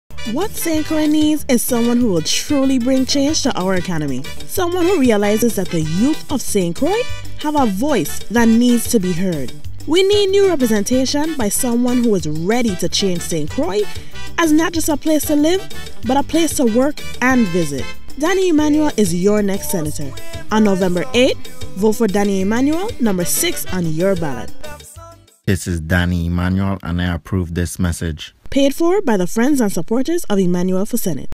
Emmanuel for Senate 2016
An ad for a client who was running for senate in the U.S. Virgin Islands.